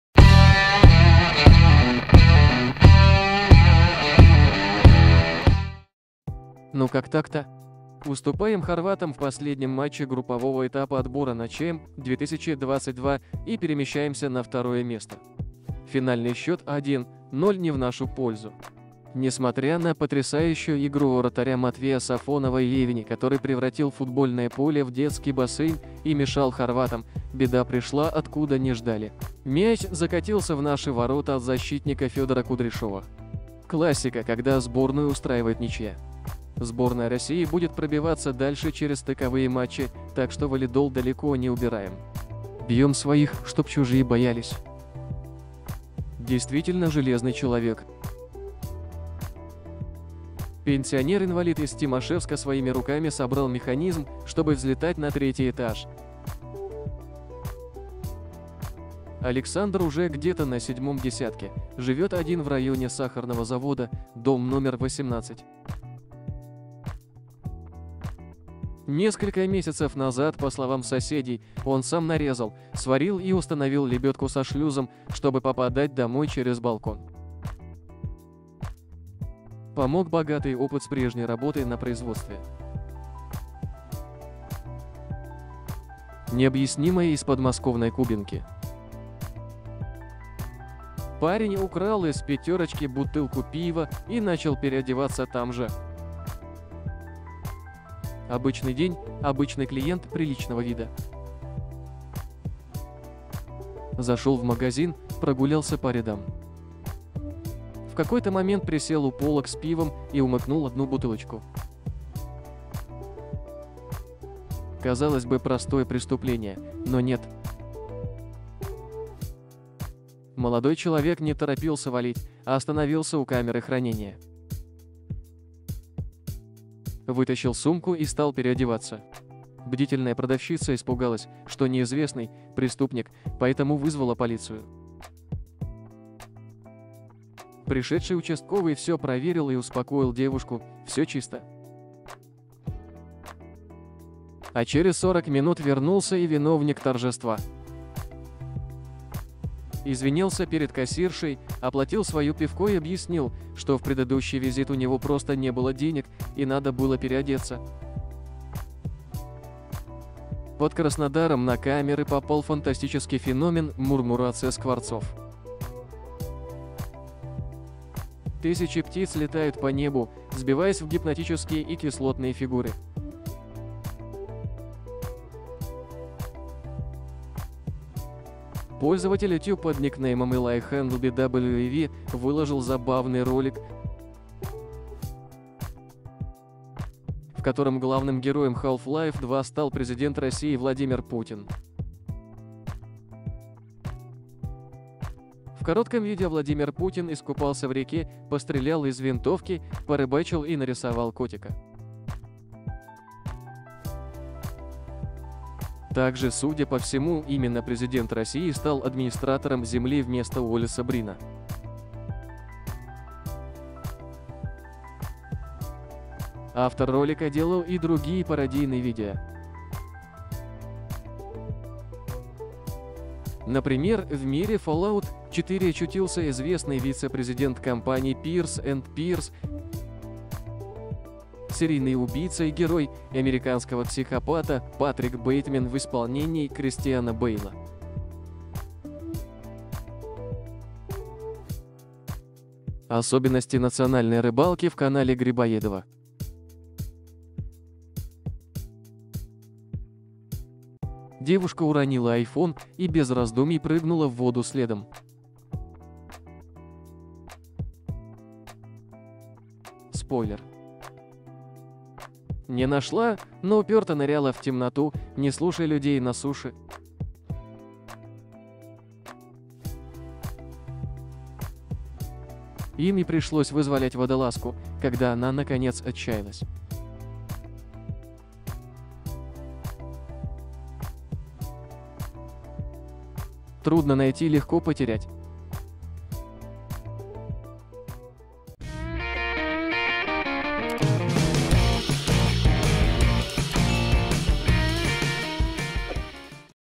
Путин идет в Half-Life. Футболисты опять... Пенсионер-каскадер. iPhone на дне реки.
Путин идет в Half-Life. Футболисты опять проиграли. Пенсионер-каскадер. iPhone на дне реки.